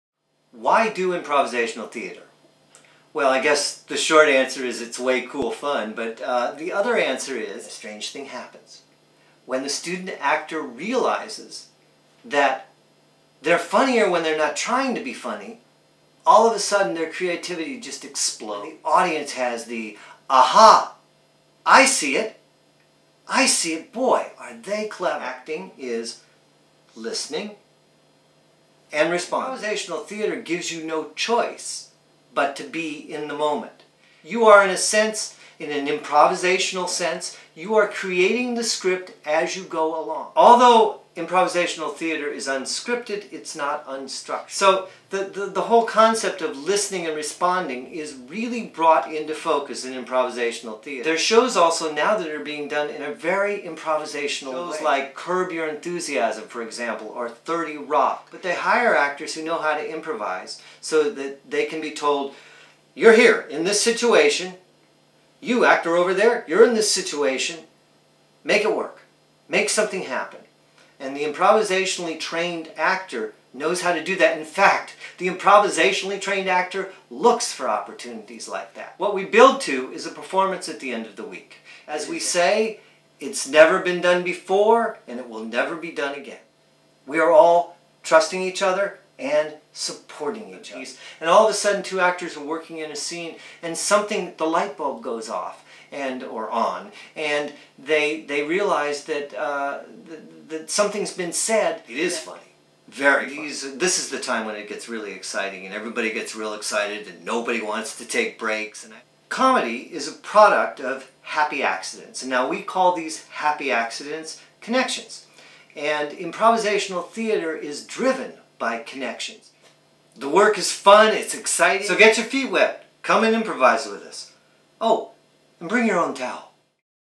US Performing Arts Comedy Improv in Washington DC
A talk with Charlie Dougherty, director of Improv Camp in Wahington DC